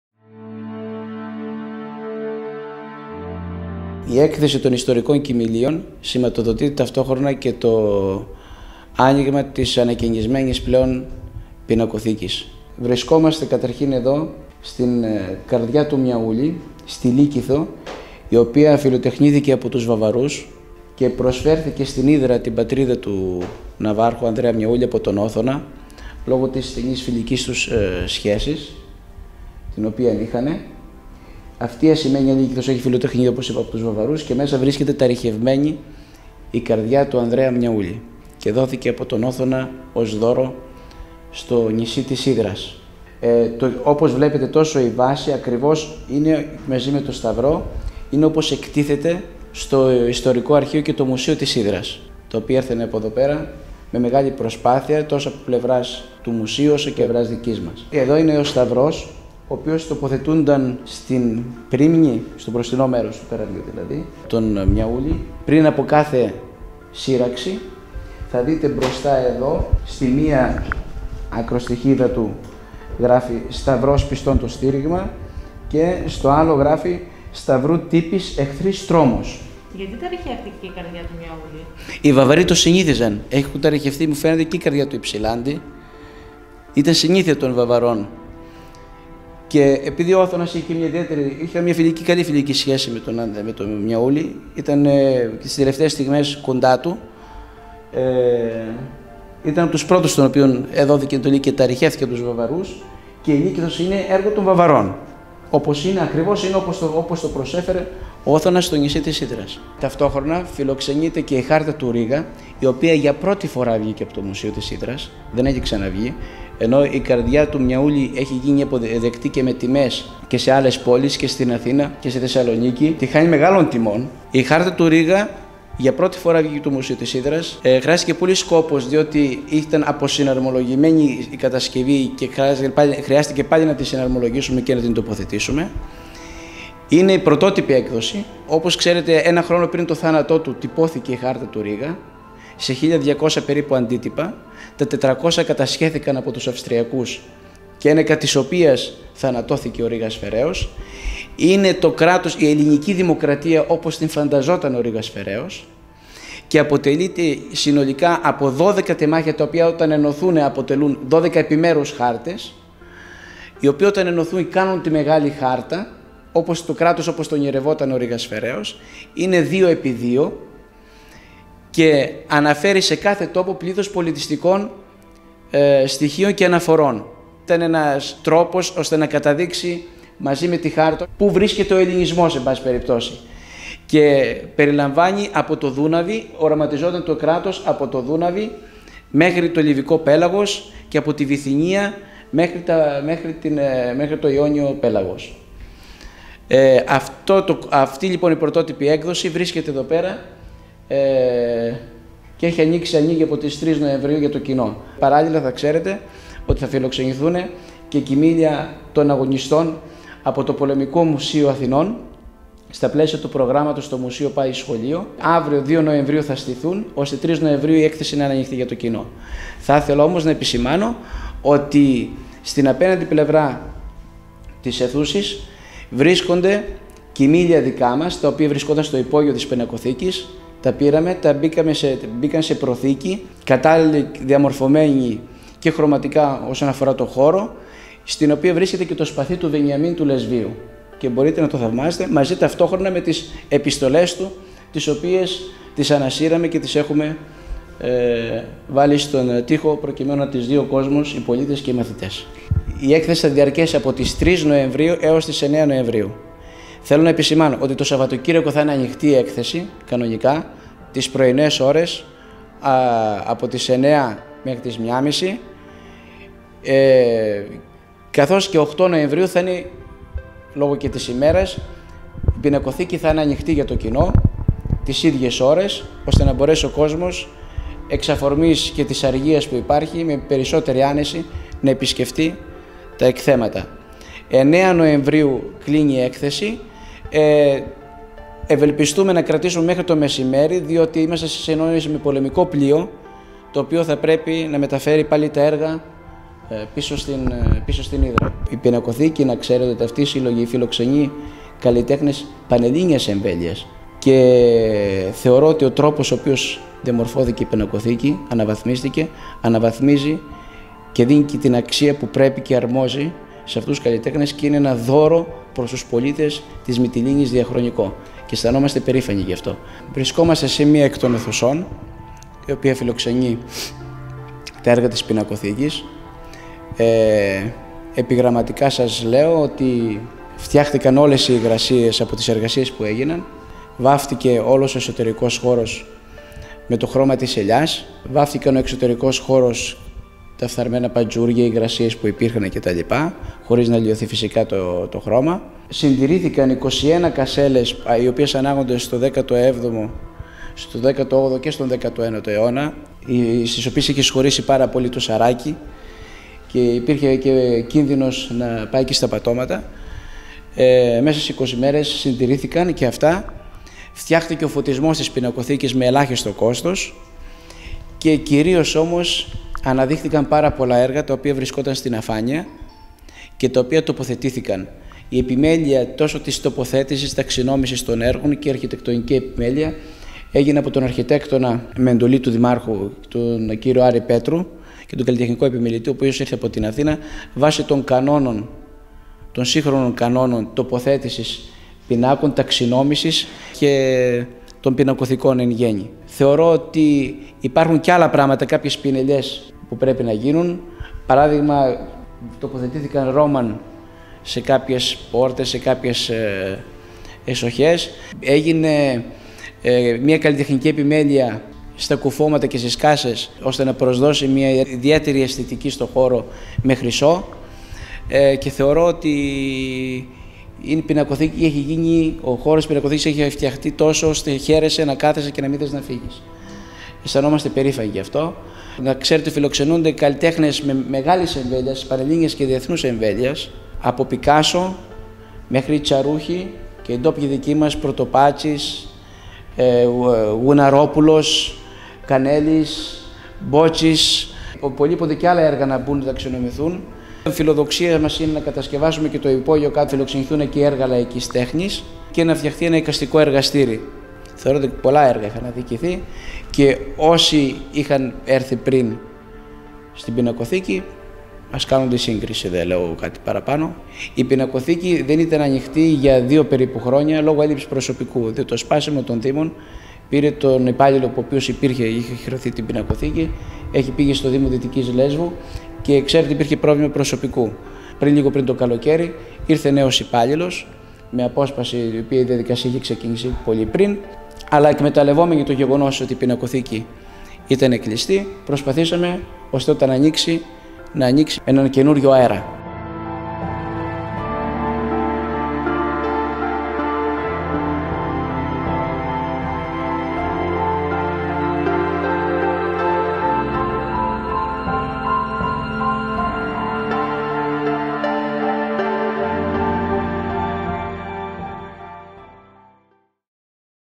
Χάρτα Ρήγα & Καρδιά Μιαούλη για πρώτη φορά στη Λέσβο
Εκτίθενται στην ανακαινισμένη Δημοτική Πινακοθήκη